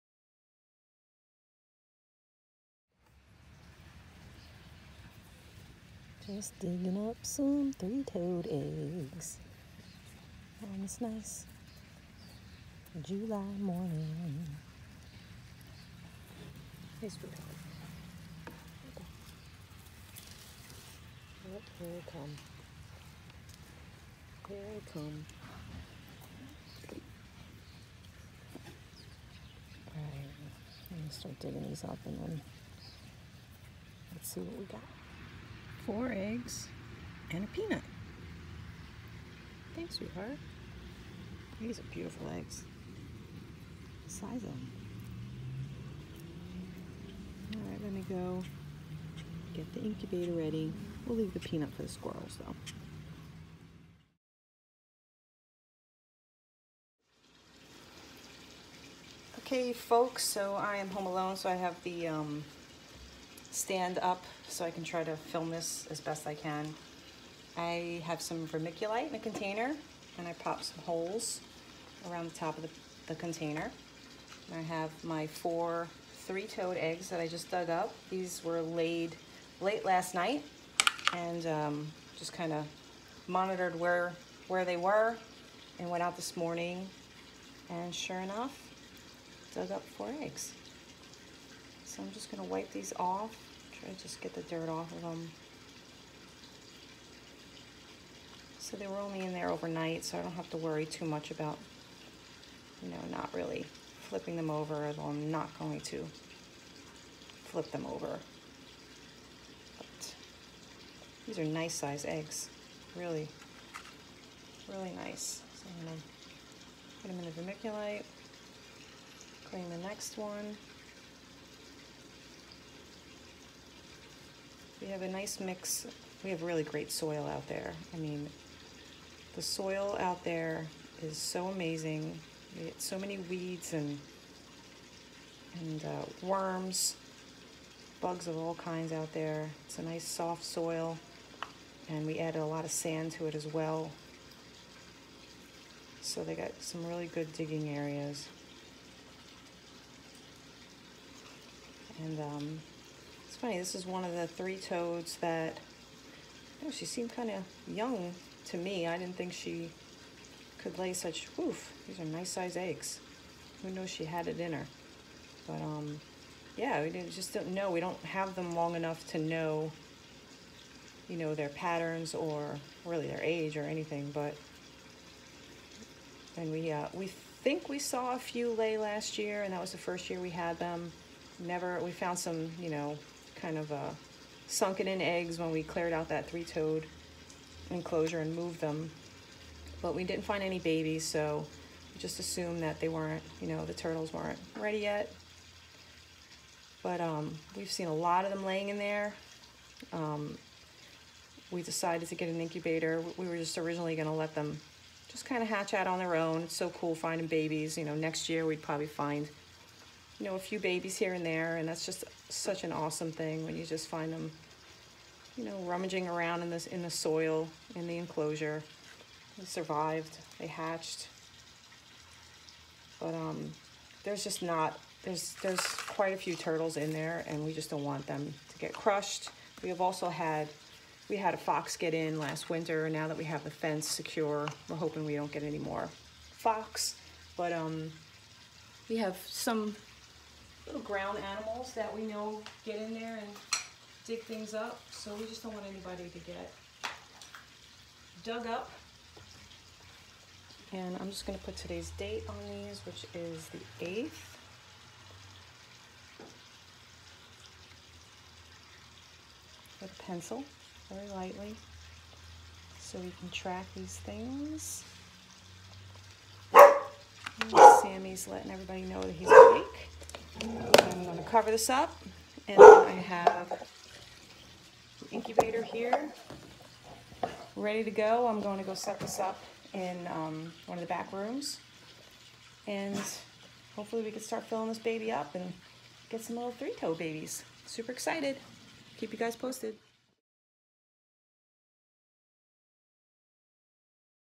How-we set up three toed box turtle eggs
We are thrilled to see our three toed box turtles thriving in their new enclosure!

4 healthy looking, hearty eggs were dug up early this July morning and set up in our incubator.

Now we wait!

Vermiculite and water are equal parts
Incubator is set at 86 degrees
We will continue to monitor the temp and count down the days!